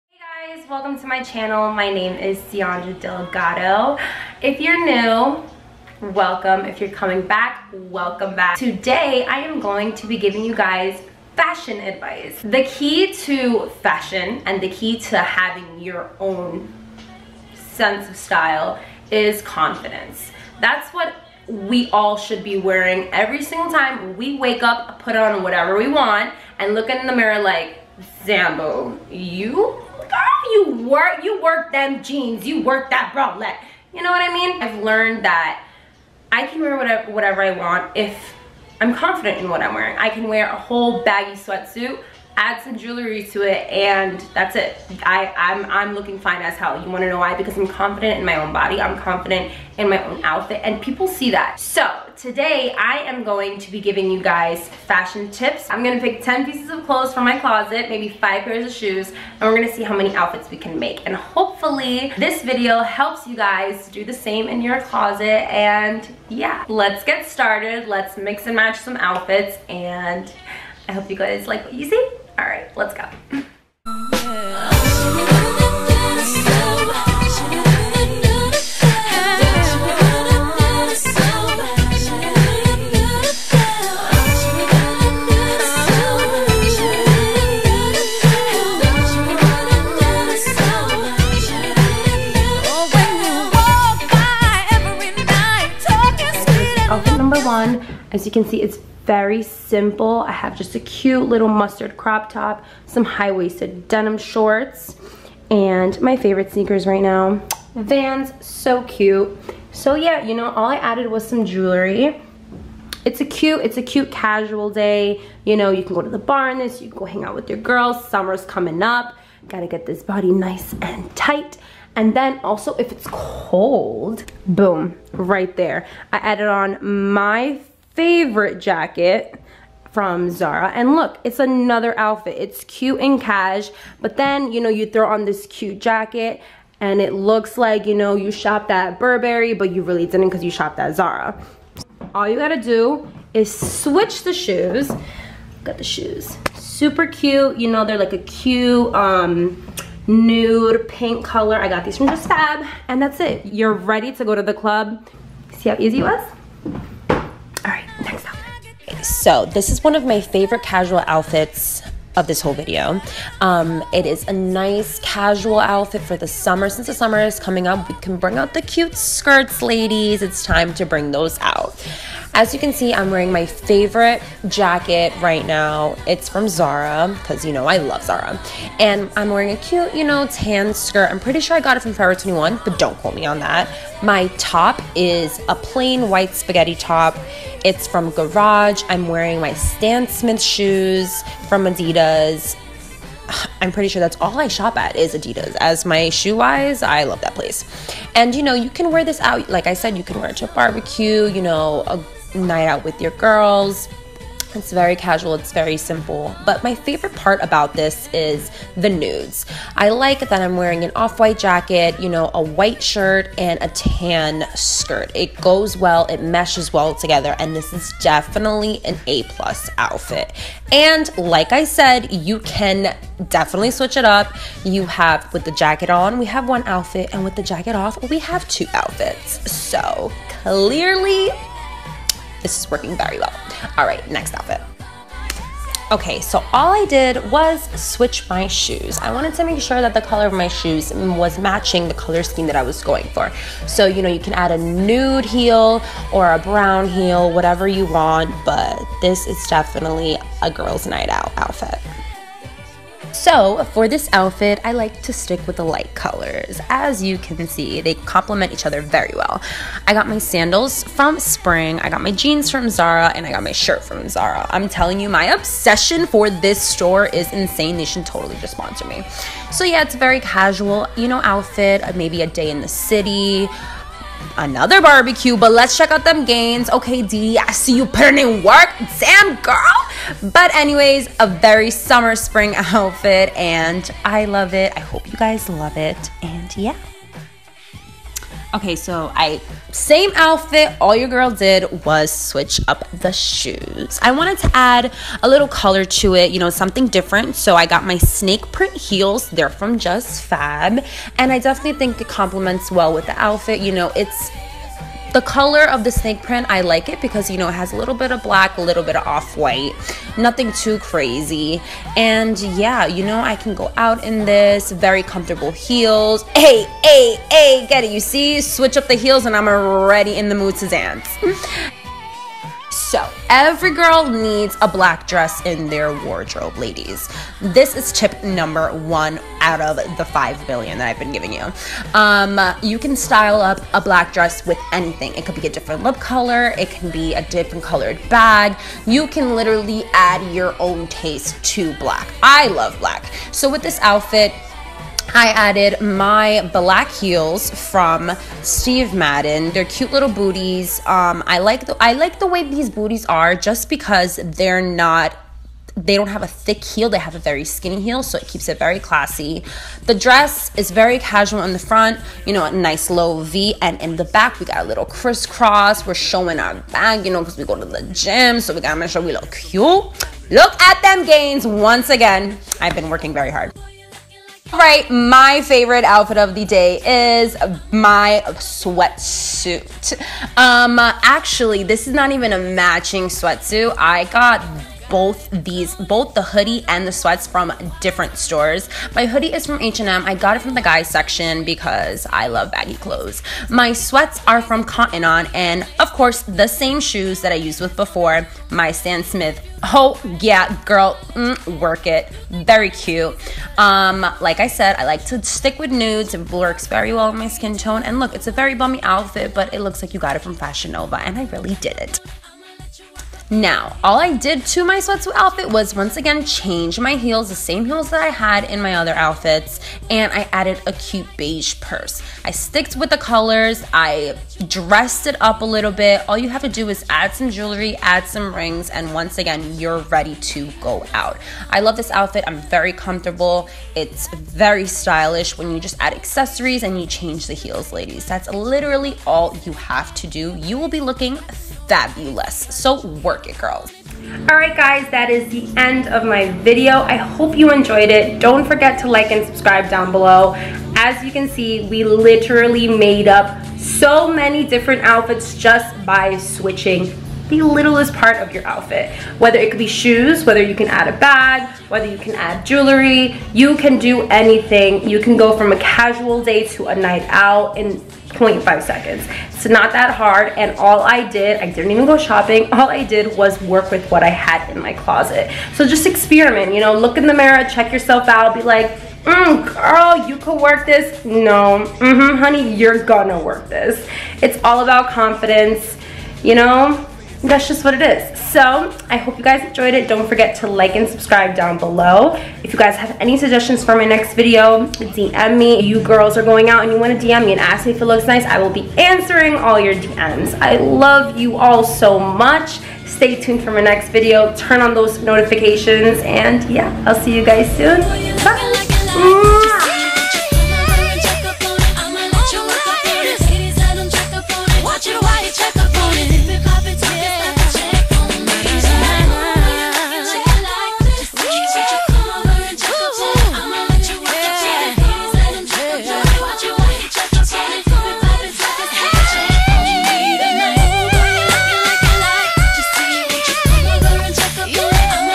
HOW TO: work with what's in your closet!
HEY BABES! A lot of you girls have been asking me how I style up my my outfits, so I decided to make a video on how to spend $0 by just working with what I have in my closet. I hope these fashion tips help you whenever your getting ready to hit the streets. Down below I've listed where I got all of my clothes just incase you liked anything I wore. See you guys in my next video :) Xoxo~

Leather Jacket,Black denim shorts,Off White Crossover Blouse,Light denim jeans: zara
orange/yellow crop top: urbanoutfitters
Black & white sneakers: vans
White Crop top: garage
Brown button up skirt: forever21
Stan smiths: adidas
Pink/nude strappy heels (first look),Snake print heels: justfab
Nude plain strappy heels(second look),Black booties: stevemadden
Sandals: spring
Black Dress: lulus
Beige Hoodie: H&M
Beige sweats: cottonon